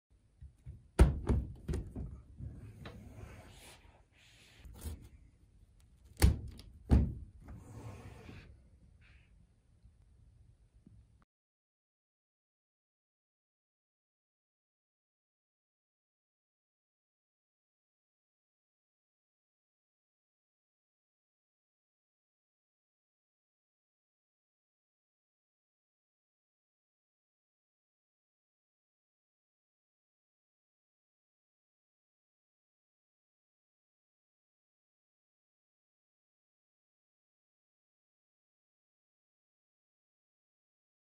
PVC Hanging Safety Shield - 36 x 24 - The Global Display Solution
Keep employees and customers safe from germs and viruses with a quick and easy sneeze guard option that can be installed in just a few minutes.  Starting at just $15.00 each.